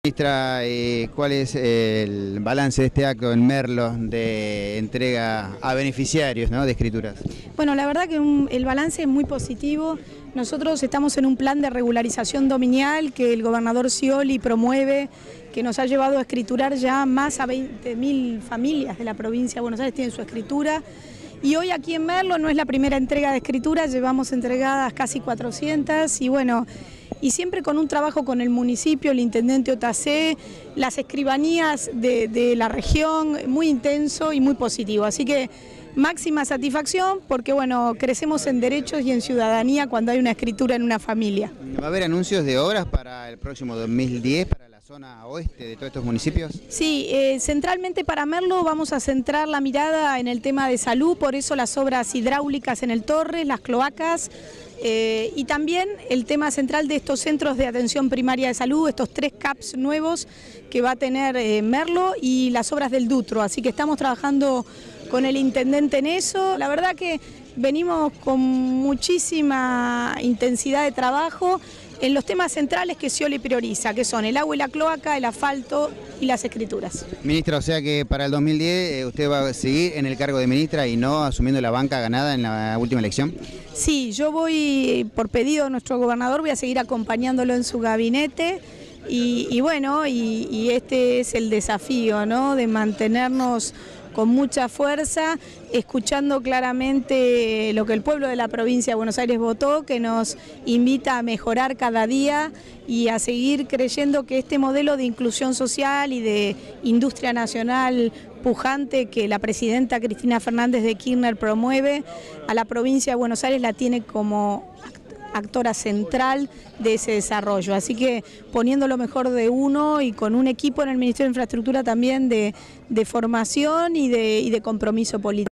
CRISTINA ALVAREZ RODRIGUEZ (programa 3/12/09)
MINISTRA DE OBRAS PÚBLICAS BONAERENSE: Entrega de escrituras para los beneficiarios de Merlo, las obras públicas en ese municipio y la confirmación que no asumirá como senadora provincial y seguirá al frente del ministerio